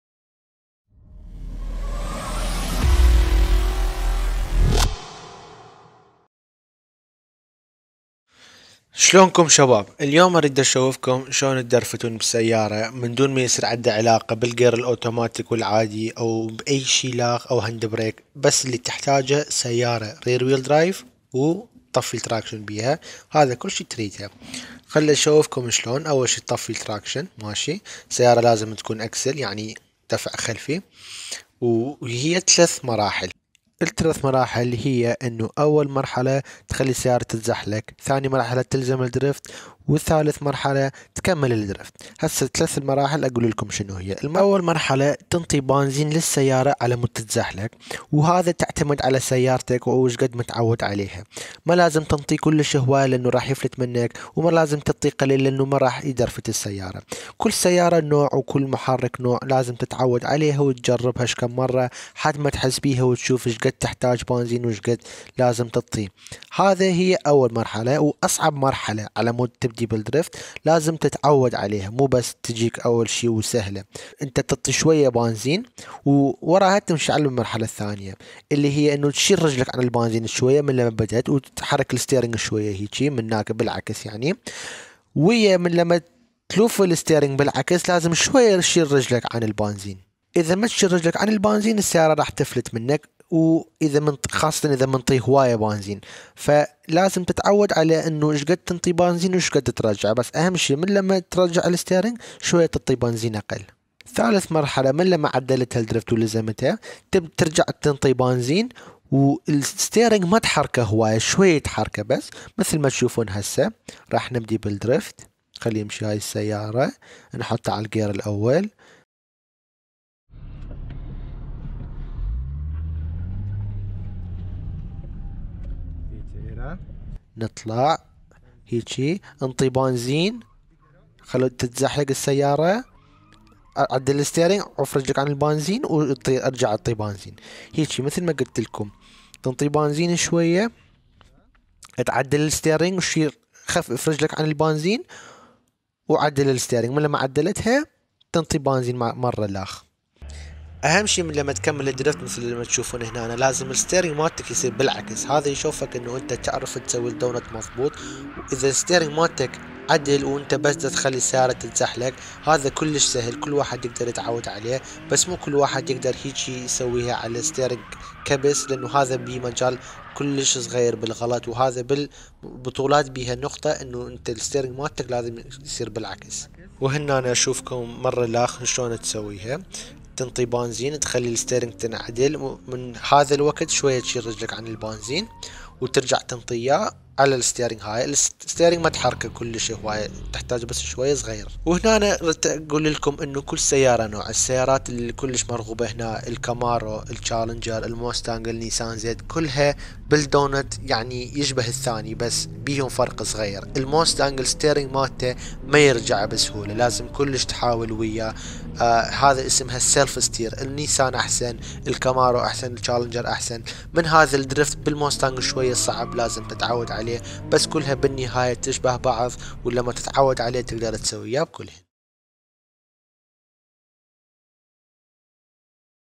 How to Drift (donuts) كيفية تعلم الدرفت
سلام و عليكم شباب. هذا الفيدو البسيط رح يساعدكم على الدرفت. الرجاء لا تجربوا بمكان عام و لا تحط حيات الناس في الخطر. اذا ساعدوني بنشر الفيديو رح اسوي فيديوهات اكثر بالعربي